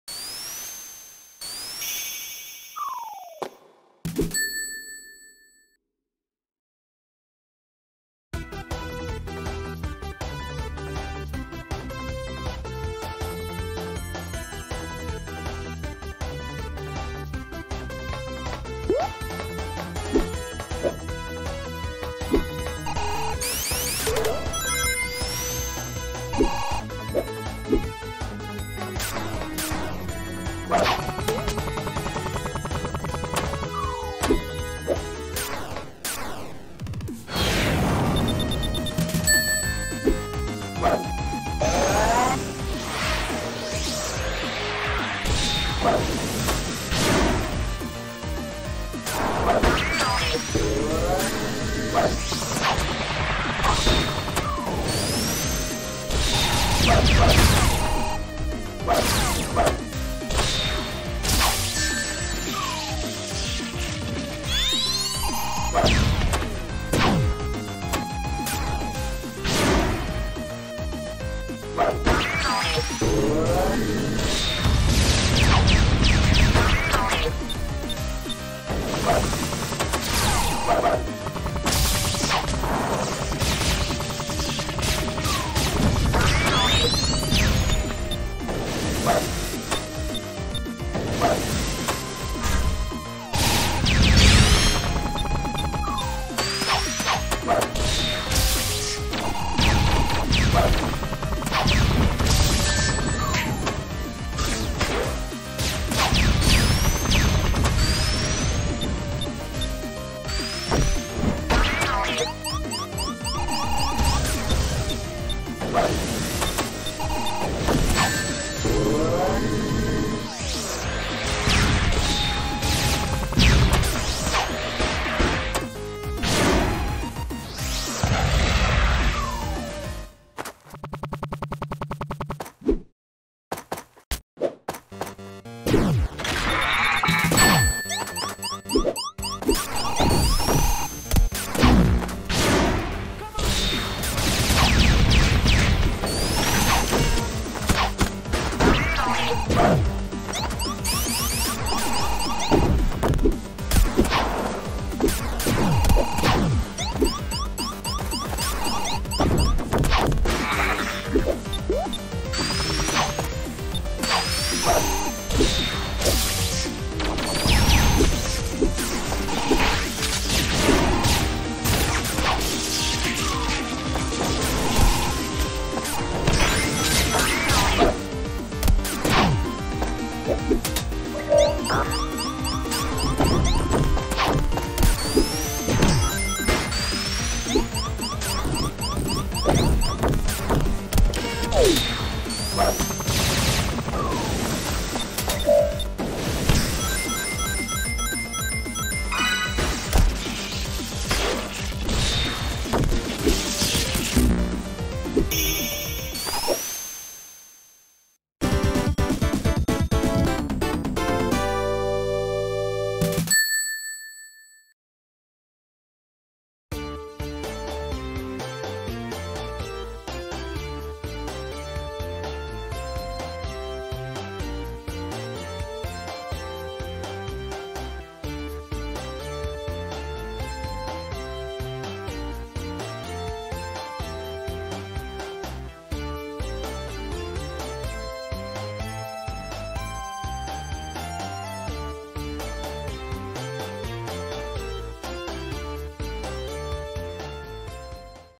Sonic 4 Episode 2 - Metal Sonic Boss Battle in CLASSIC style [Animation]
For a long time I was thinking about remaking the battle with Metal Sonic into a classic style.
Of course with a small part of my own imagination that you love this much.
Music

Sprites